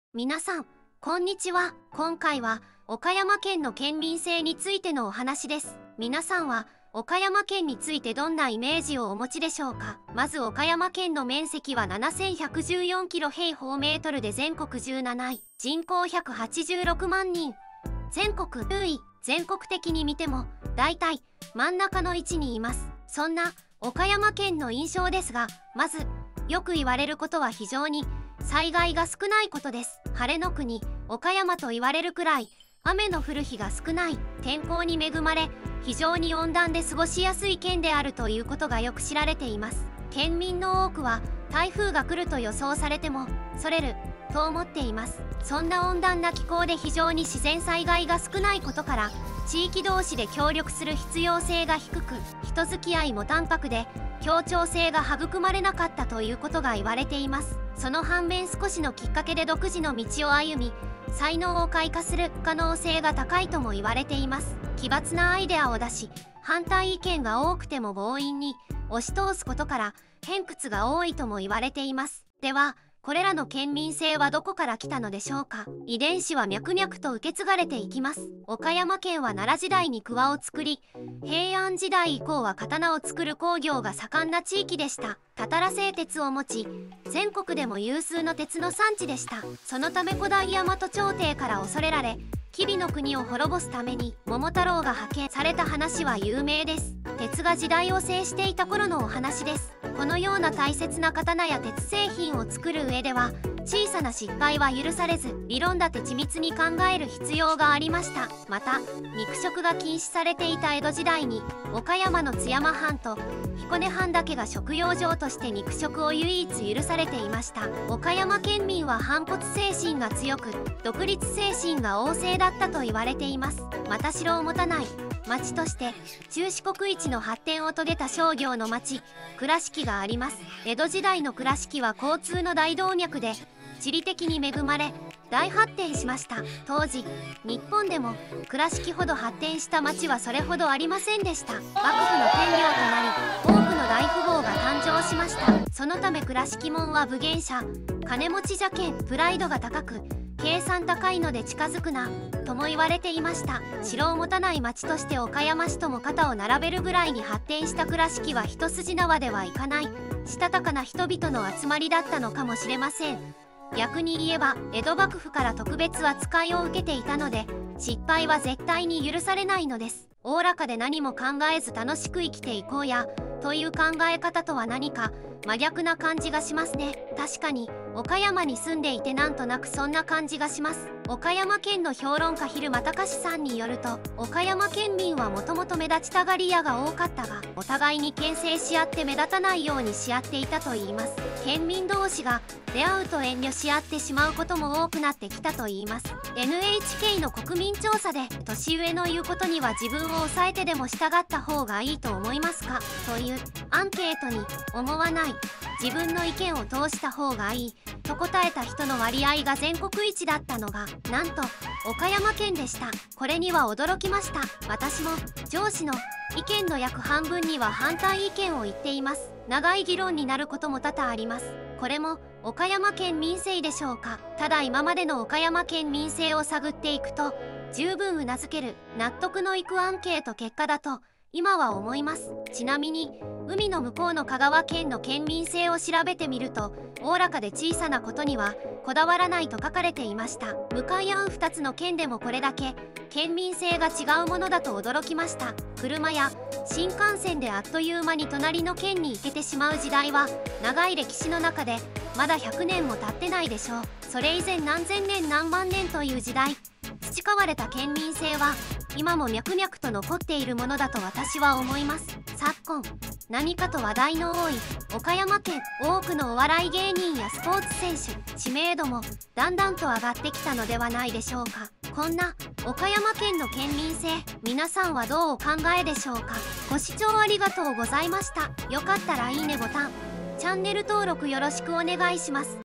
岡山県民性 驚きの真実 反骨精神旺盛
今回は、岡山県の県民性についてのお話です。 皆さんは 岡山県についてどんなイメージをお持ちでしょうか。 まず 岡山県の面積は 7114 km2で全国17位。人口186万人 全国 20位。
 全国的に見ても だいたい 真ん中の位置にいます。 そんな 岡山県の印象ですが、 まず、 よく言われることは非常に 災害が少ないことです。 晴れの国岡山と言われるくらい、雨の降る日が少ない。 天候に恵まれ 非常に温暖で過ごしやすい県であるということがよく知られています。
県民の多くは、台風が来ると予想されても、  ’それる’と思っています。
そんな 温暖な気候 で非常に自然災害が少ないことから、 地域同士で協力する必要性が低く、人付き合いも 淡泊で、協調性が育まれなかったということが言われています。
 その半面 少しのきっかけで独自の道 を歩み、 才能を開花する可能性が高いとも言われています。
 奇抜なアイデアを出し、 反対意見が多くても強引に 押し通すことから、偏屈が多いとも言われています。
 ではこれらの県民性はどこから来たのでしょうか。 遺伝子は脈々と受け継がれていきます。
 岡山県は奈良時代に鍬を作り、 平安時代以降は刀を作る工業が盛んな地域でした。 たたら製鉄 を持ち全国でも有数の鉄の産地でした。 そのため 古代大和朝廷から恐れられ 吉備の国を滅ぼすために 桃太郎が派遣 された話は有名です。 鉄が時代を制していた頃のお話です。
 このような大切な 刀や 鉄製品を作る上では小さな失敗は許されず 、理論立て緻密に考える必要がありました。
 また  肉食が禁止されていた江戸時代に、  岡山の津山藩と 彦根藩だけが食養生として肉食を唯一許されていました。
岡山県民は反骨精神がすごく、 独立 精神が旺盛だったと言われています。
 また城を持たない 城下町として 中四国1の発展を遂げた商業の町、 倉敷があります。 江戸時代の倉敷は交通の大動脈で、地理的に恵まれ、   大発展しました。 当時 日本でも 倉敷ほど発展した町はそれほどありませんでした。 幕府の天領となり、多くの大富豪が誕生しました。 そのため 倉敷 者は分限者（金持ち）じゃけん、 プライドが高く 計算高いので近づくな とも言われていました。 城を持たない街として 岡山市 とも 肩を並べるぐらいに発展した 倉敷は一筋縄ではいかない、 したたかな人々の集まりだったのかもしれません。、逆に言えば 江戸幕府から特別扱いを受けたいたので 失敗は絶対に許されないのです。
おおらかで何も考えず楽しく生きていこうや という考え方とは何か 真逆な感じがしますね。
確かに 岡山に住んでいてなんとなくそんな感じがします。

 岡山県の評論家 昼間たかしさんによると、 岡山県民は元々目立ちたがり屋が多かったが お互いに牽制しあって目立たないようにし合っていたと言います。 県民同士が 出会うと遠慮しあってしまうことも多くなってきたと言います。

 NHK の国民調査で年上の言うことには自分を抑えてでも従った方がいいと思いますか

 という アンケートに 思わない 自分の意見を通した方がいい と答えた人の割合が全国1だったのが
なんと 岡山県でした。 これには驚きました。
私も 上司の 意見の約半分には反対意見を言っています。 長い議論になることも多々あります。
 これも 岡山県 民性でしょうか。

 ただ今までの 岡山 県民生を探っていくと 十分 頷ける 納得のいく アンケート結果だと今は思います。

ちなみに 海の向こうの香川県の県民性を調べてみると おおらかで小さなことには こだわらないと書かれていました。 向かい合う2つの県でもこれだけ 県民性が違うものだと驚きました。

 車や 新幹線であっという間に隣の県に行けてしまう時代は、長い歴史の中で まだ100年も経ってないでしょう。それ以前何千年何万年という時代、 培われた県民性は 今も 脈々と残っているものだと私は思います。

音声：VOICEBOX：四国めたん